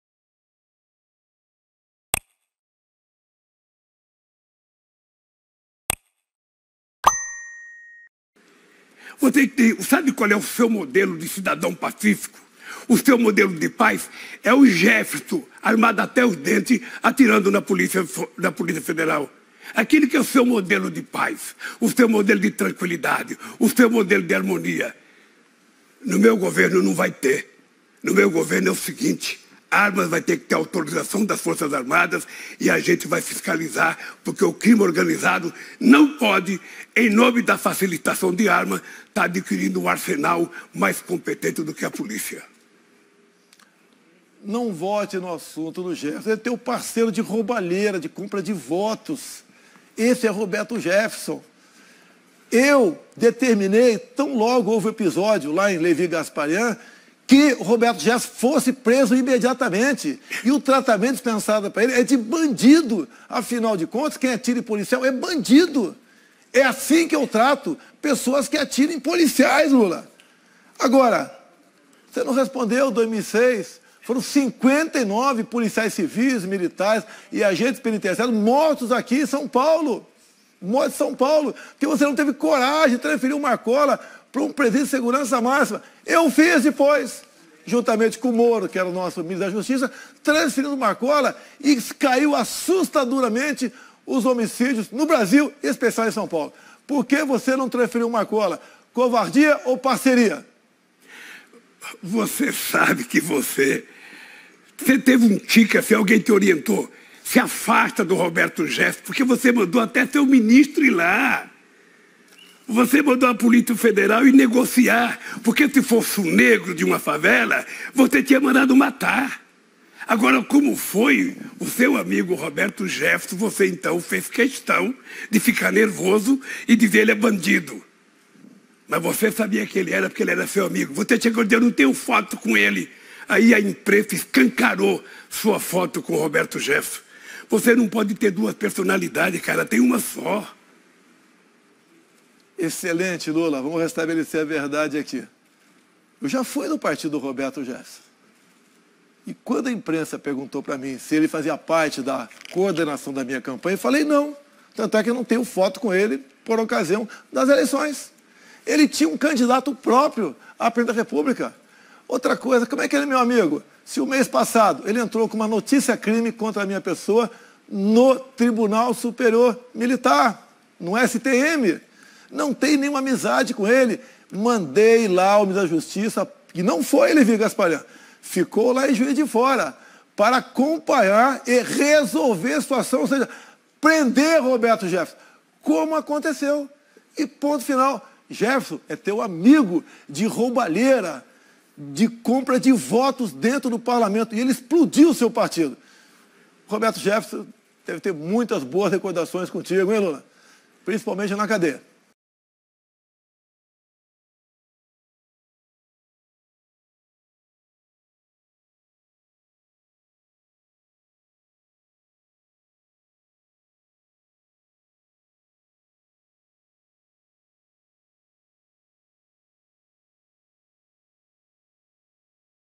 É seu parceiro e amigo de roubo e deve lembra da cadeia, diz Bolsonaro sobre Lula e Jeferson
✔Comente de qual sua cidade e estado.

HotnotBR - Noticias, famosos e TV
Em Deus confiamos.